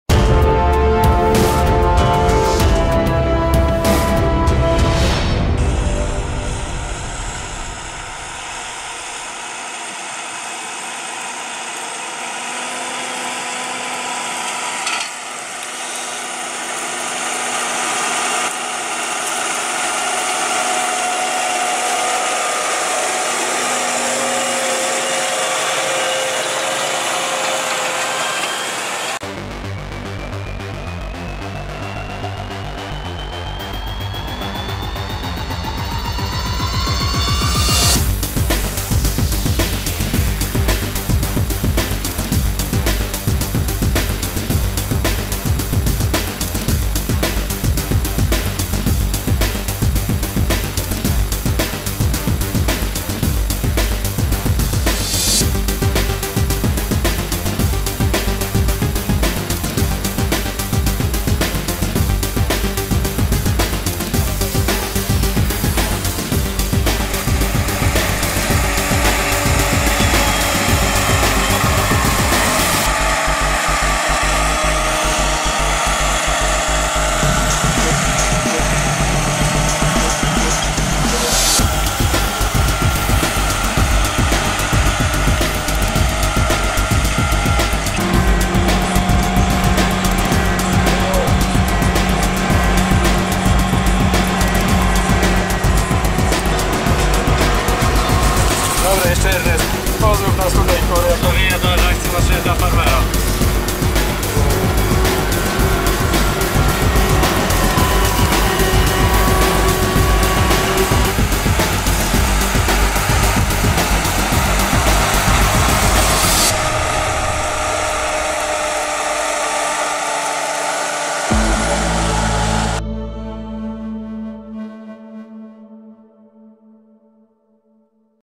New Holland T7.270 w naszym obiektywie (Erni w akcji)
New Holland T7.270 Auto Command Tier 4B z kultywatorem Pottinger Synkro 4030. Pozdrowienia dla Marka właściciela ciągnika i operatora Ernesta. Więcej o prezentowanych maszynach na